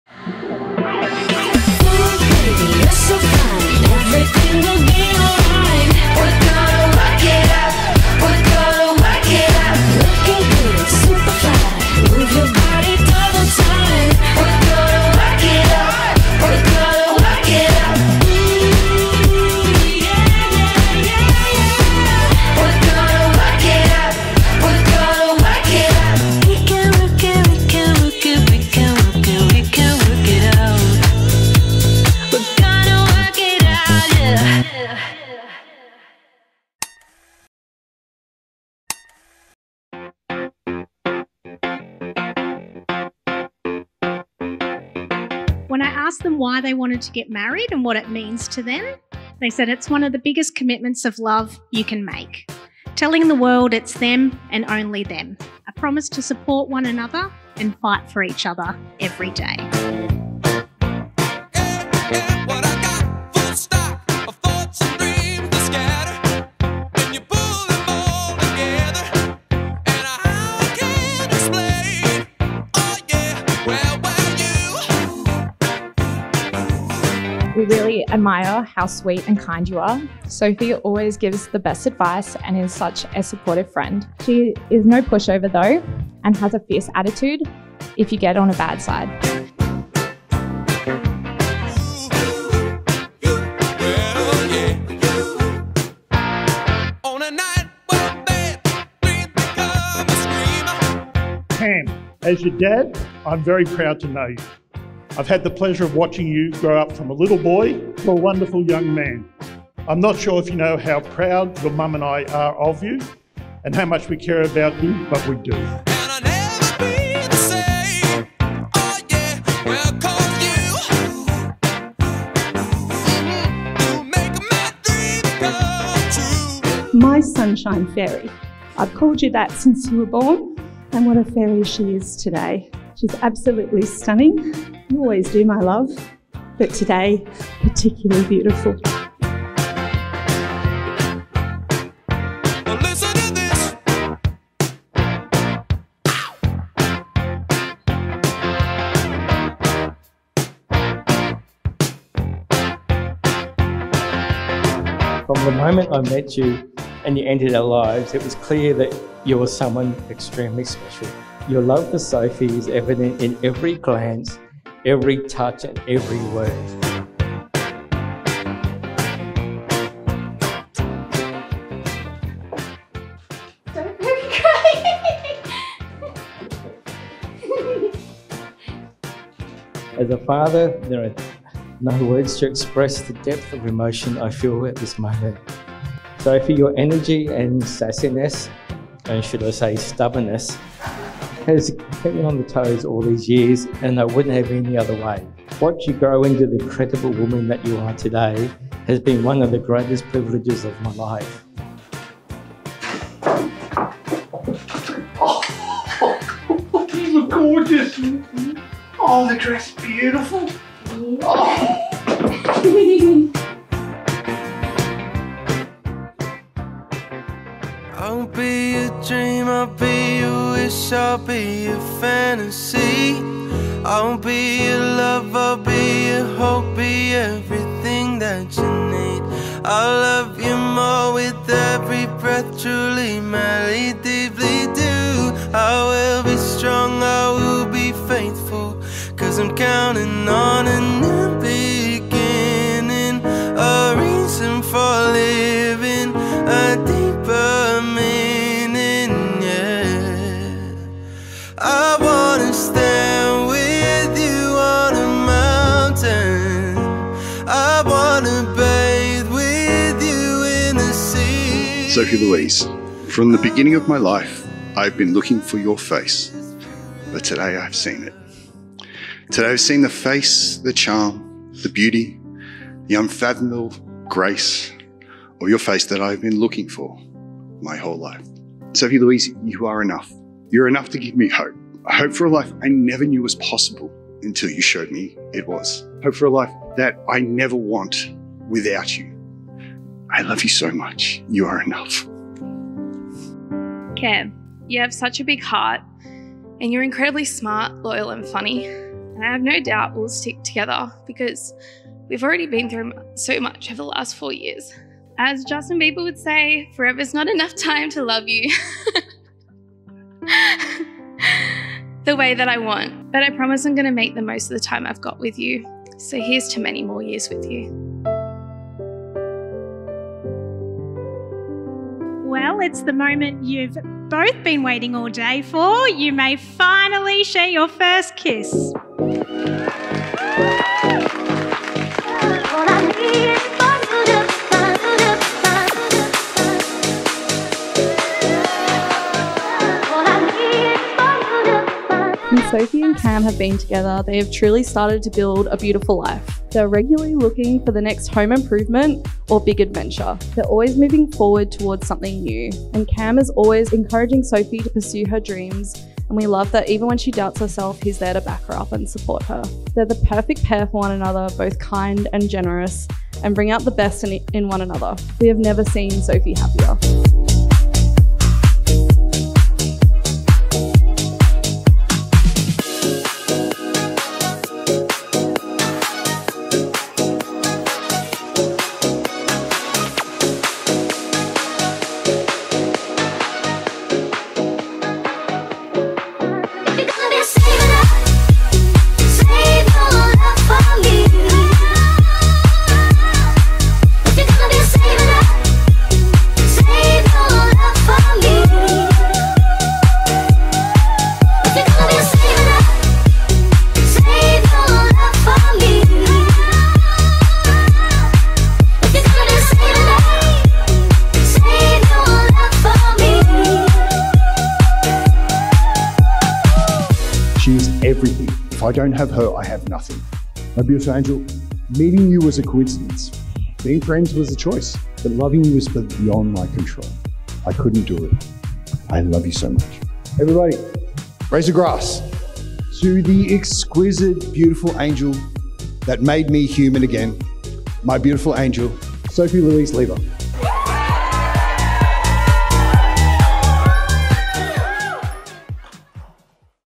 Sophie + Cameron // KIMO ESTATE // Sydney Wedding Videography // MK PIXELS
Capturing Sophie and Cameron's magical day at Kimo Estate was an absolute delight! 💍💕

Venue : @kimoestate
Photo & Video : @mkpixels.wedding

Book Your Day: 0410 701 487

Music Lic : MB01732B0WWVAAT